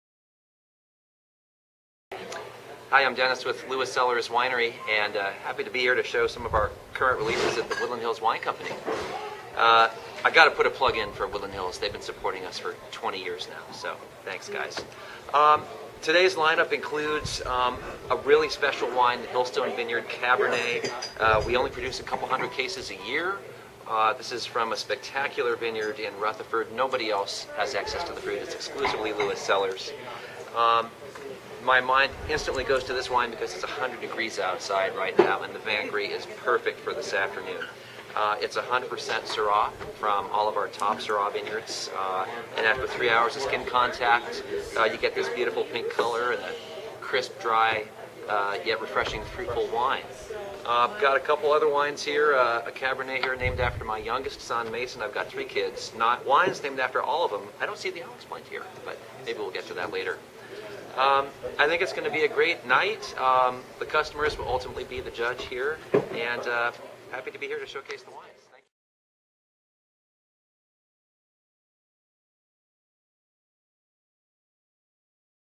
Dennis Bell, Lewis Cellars 9/14/12
On Friday evening Sept 14th Dennis Bell was in the house to pour a selection of his new releases and vintage offerings.